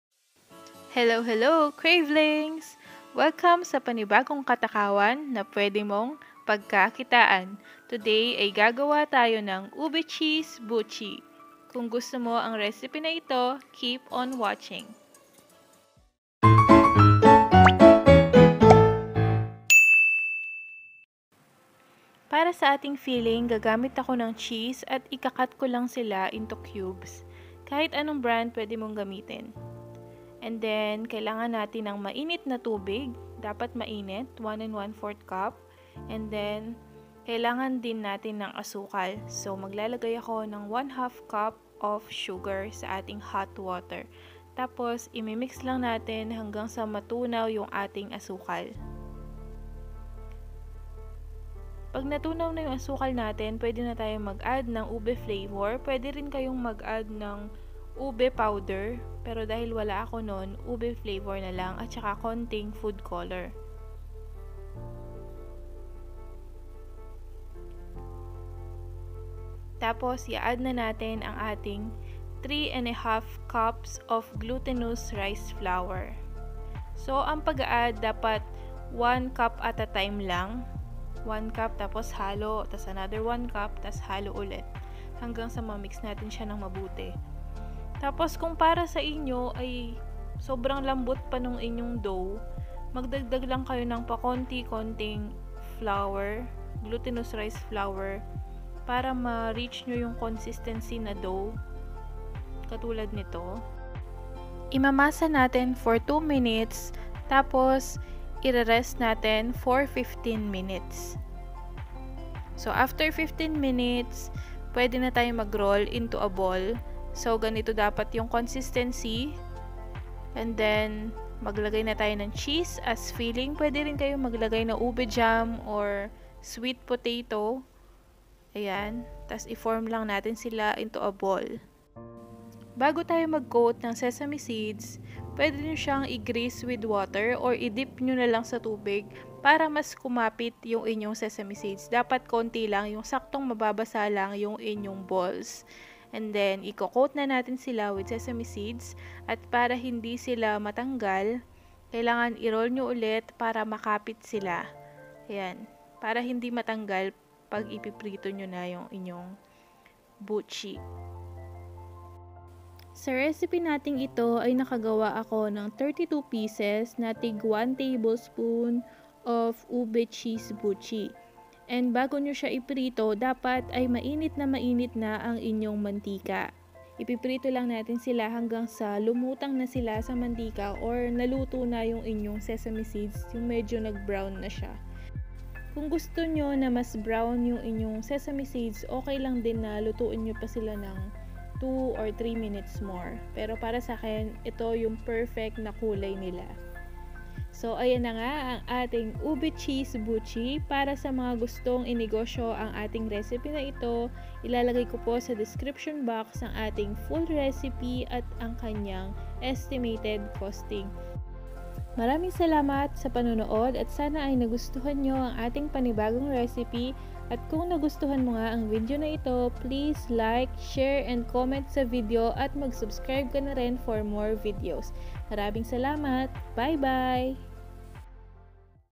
150 PUHUNAN! Matutong gumawa ng Ube Cheese Buchi, Simpleng Sangkap na Pwedeng Pagkakitaan
Matutong gumawa ng Ube Cheese Buchi, Simpleng Sangkap na Pwedeng Pagkakitaan
SESAME BALLS RECIPE
BUCHI CHEESE
BOCHI BUTCHI BOTCHI

Ingredients:
1 1/4 cup Hot Water
1/2 cup Sugar - 16php
1 tsp Ube Flavor - 2php
Food Color - 1php
3 1/2 cups Glutinous Rice Flour - 30php
1 cup Sesame Seeds - 20php
Cheese 200g - 37php

Cooking Oil - 20php
Utilities - 20php

Yield: 32pcs

TOTAL: 146PHP
COST / YIELD: 4.5PHP
SRP: 50php to 60php per 6 pcs